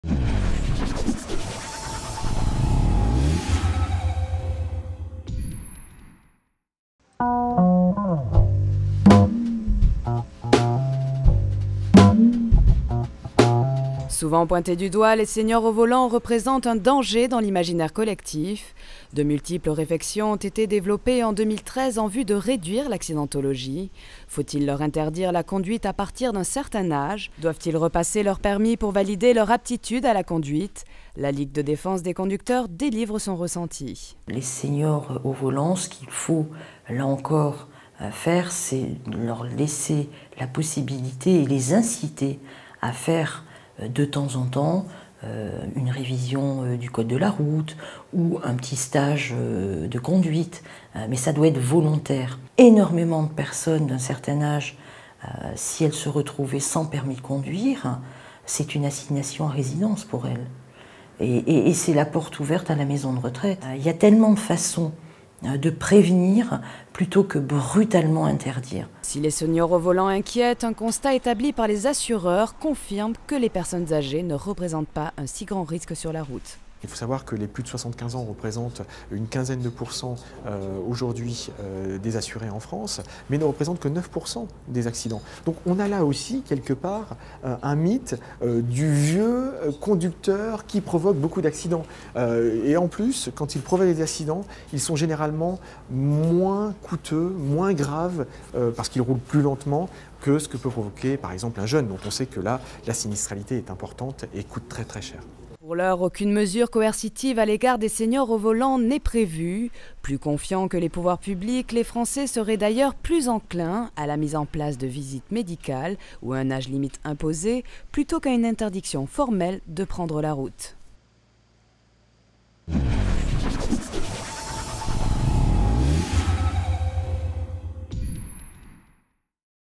Seniors au volant : Faut-il leur interdire de conduire ?
la rédaction de News Assurances s'est intéressé à la question des seniors au volant.

Depuis quelques années, les pouvoirs publics sondent régulièrement les experts et la population, dans le but d'établir des mesures préventives et garantir la sécurité de chacun. Doit-on s'attendre à voir apparaitre une interdiction de conduite pour les personnages âgées ? Représentent-t-elles un risque si grand ?